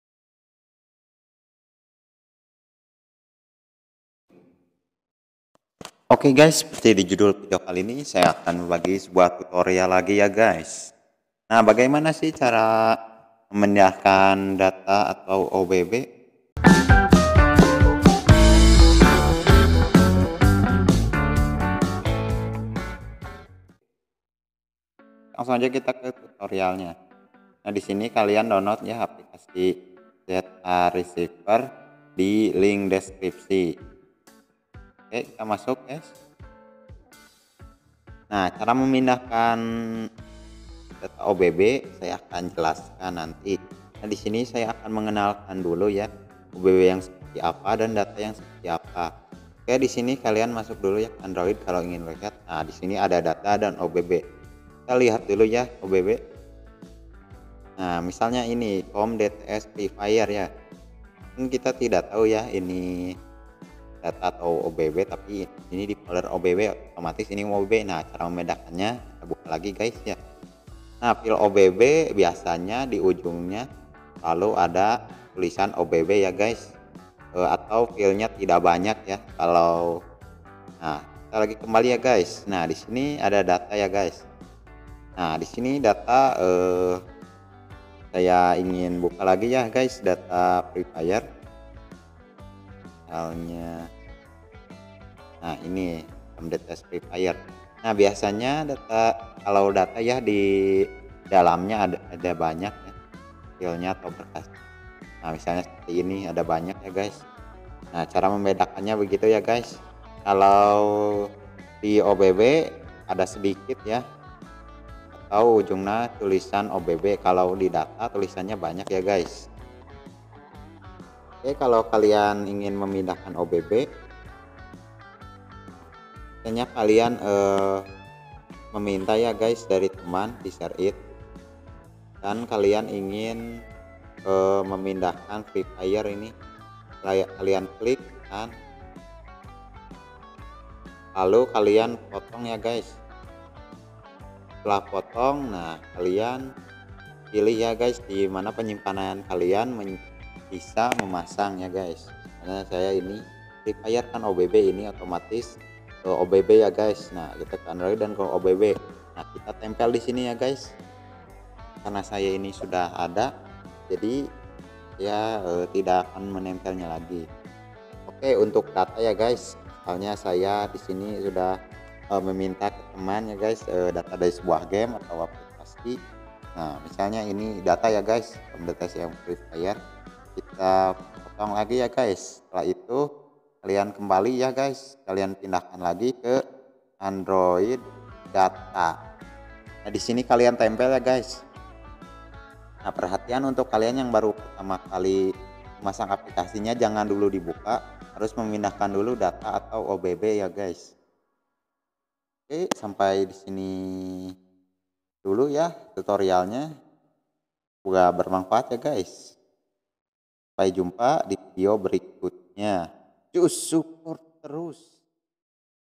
Cara Memasang Data + Obb Aplikasi - ZArchiver
Ok di vidio kali ini saya akan memberikan tutorial tentang  memindahkan file obb dan data aplikasi, Karena mungkin dari kalian belum ada yang bisa cara memindahkannya, Berikut ini trik dan tips sangat mudah dan cepat untuk pindahkan data atau obb.

Kalian bisa Download aplikasi ZArchiver agar memudahkan, atau kalian juga bisa lakukan di berkas tanpa perlu Download aplikasi ini.

Kalau ada yang belum paham silakan komentar di bawah⤵️📝